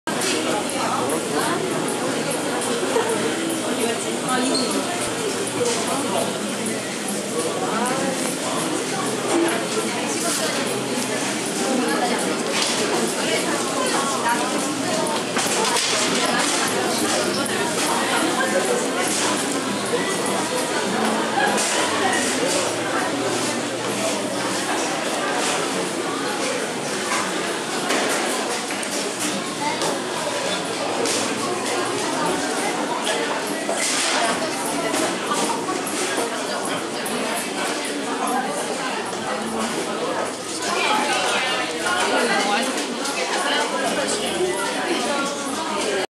passion5
A short walk on "Passion 5 Bakery" done a Pentax K-x with a 10mm fisheye lens.
Passion 5 is a very good and expensive bakery located in Iteawon, Seoul, South Korea.
Highly recommended.